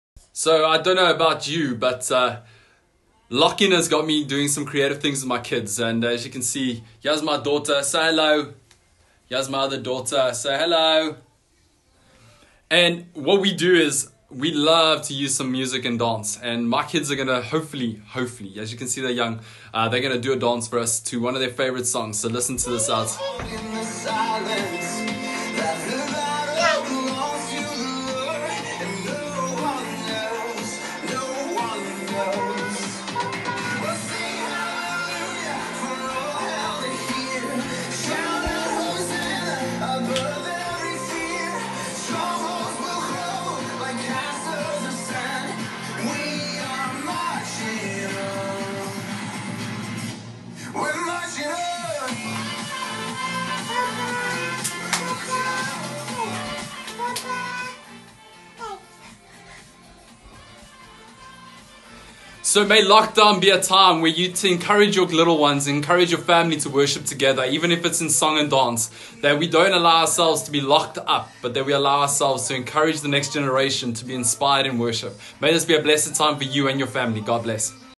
SA Lockdown day 20 with Garth Langeveld - Worship together as a family
Garth Langeveld, from Doxa Deo Langeberg, shares a short video of how he spends time with his kids and make worship fun as a family!